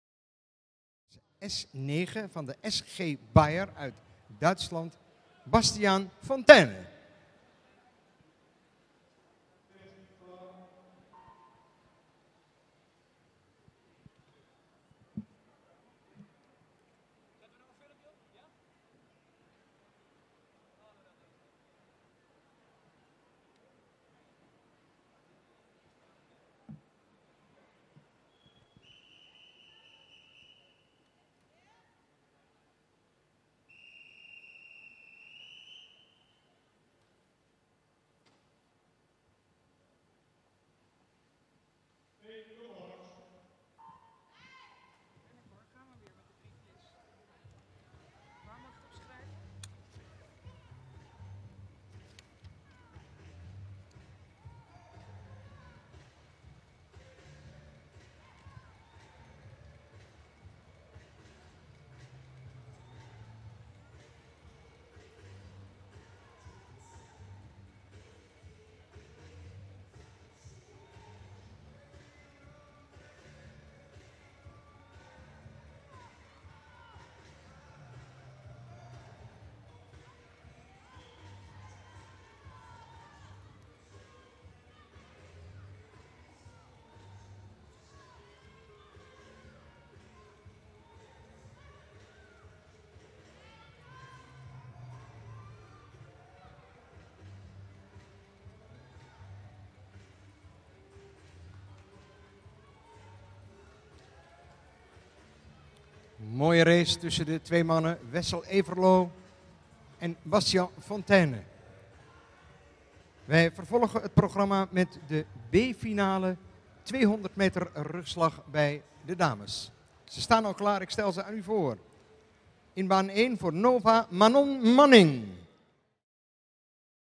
#8-Swim-off - Men 100 m Freestyle - Eindhoven Swim Cup 2015 | Zwemmen
Eindhoven Swim Cup 2015 - #8-Swim-off - Men 100 m Freestyle